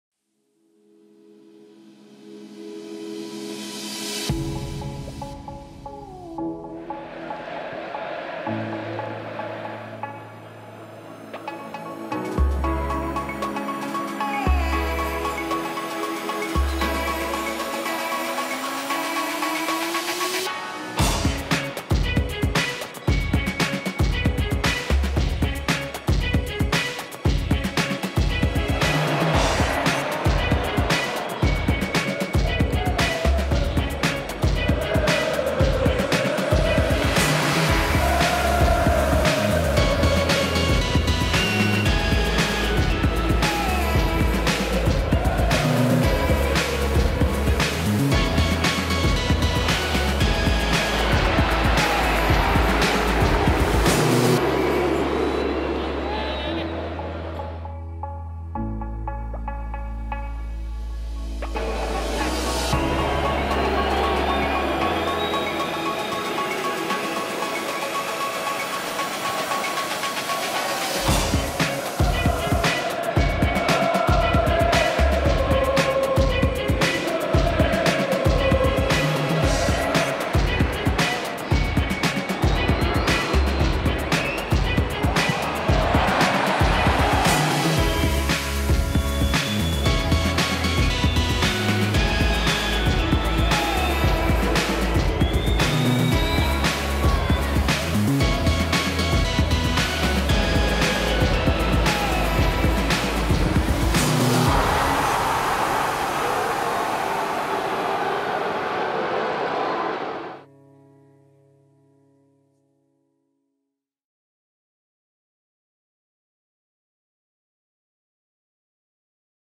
Spotlight on Leandro Trossard
We put the spotlights on Leandro Trossard during the NationsLeague game between Wales and Belgium.